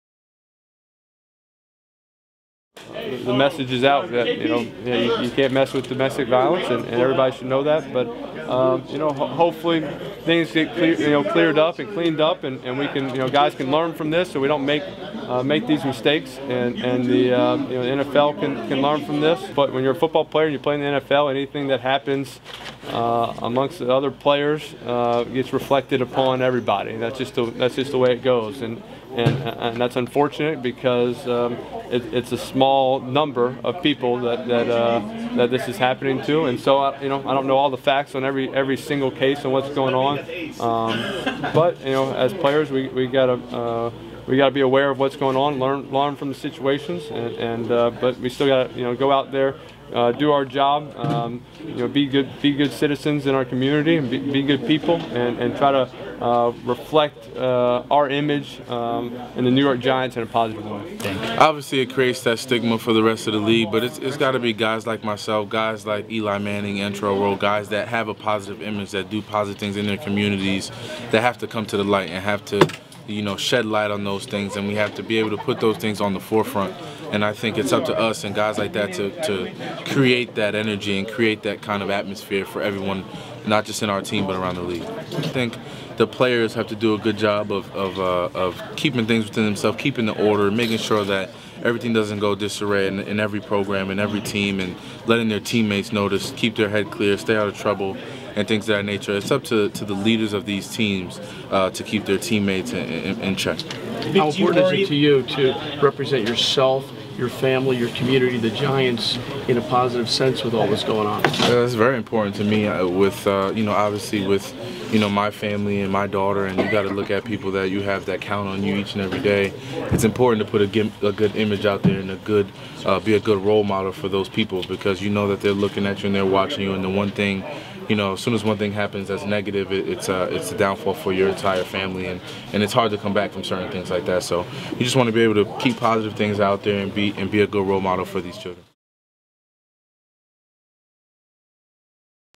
Giants Manning and Cruz reflect on the state of the NFL and how they can make a difference
New York Giants QB Eli Manning and WR Victor Cruz remark on the state of the NFL in light of domestic violence and what they can do to make a difference. ‘... guys can learn from this so we don’t make these mistakes and the NFL can learn from th